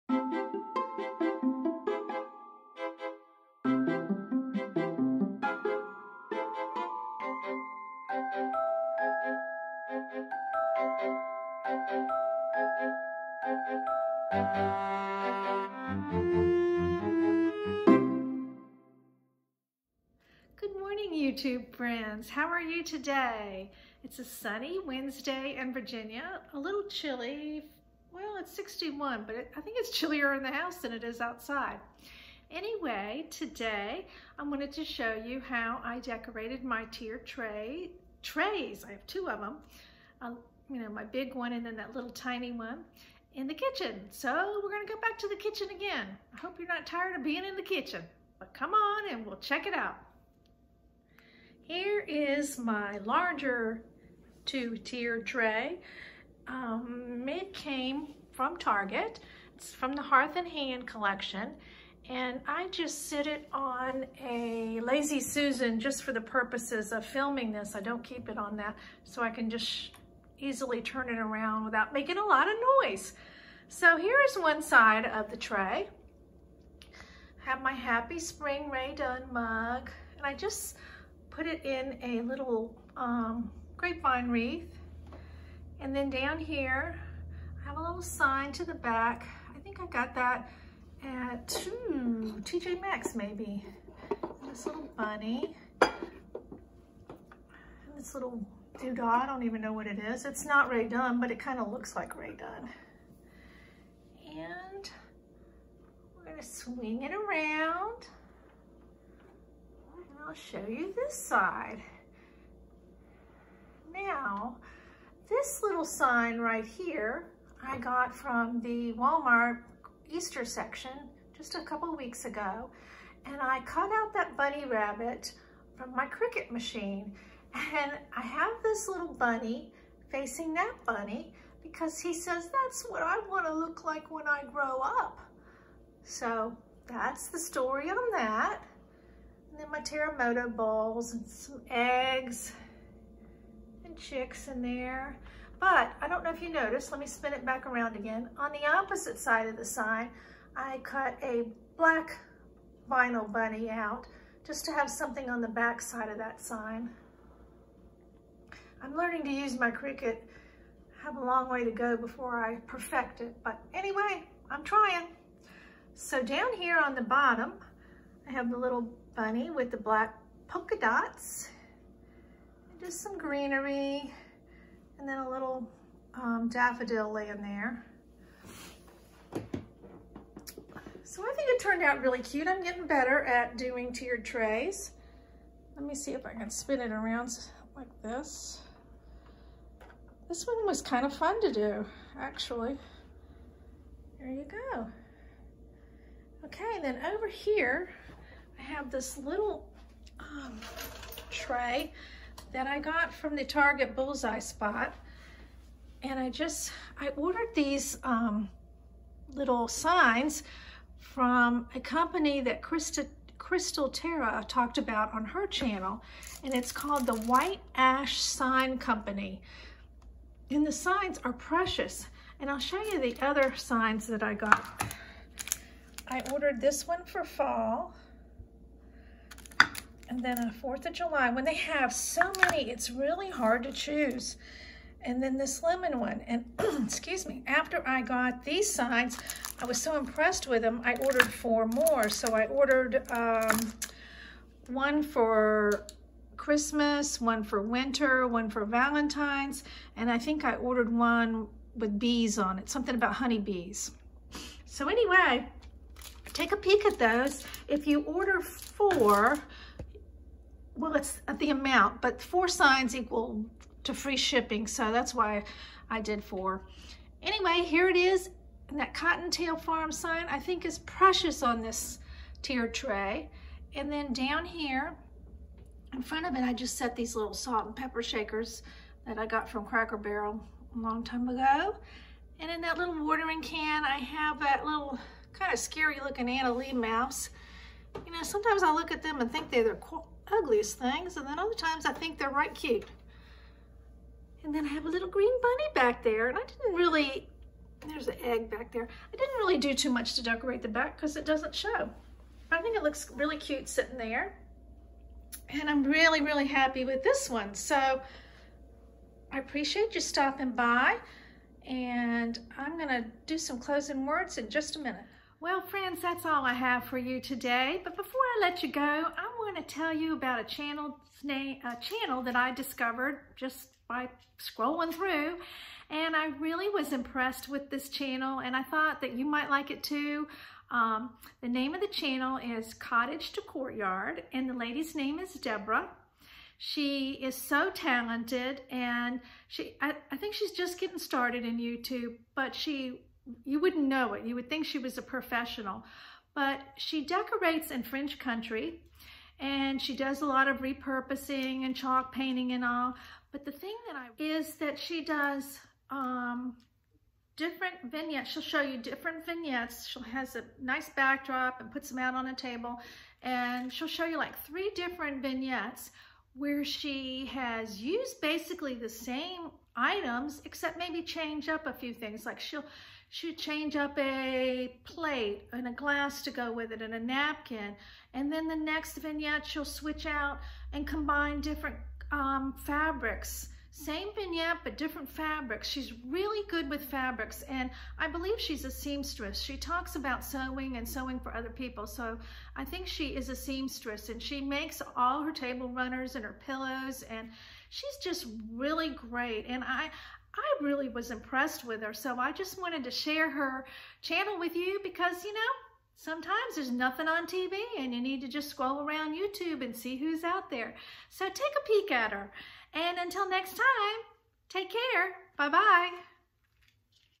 Easter Tiered Trays🐇🌷🥕
Hi Sweet Friends, Today I wanted to share how I decorated my tiered trays for Easter. I hope you like how I dressed the trays up! Thanks for stopping by!easter2020 tieredtray#

Link for the wooden signs: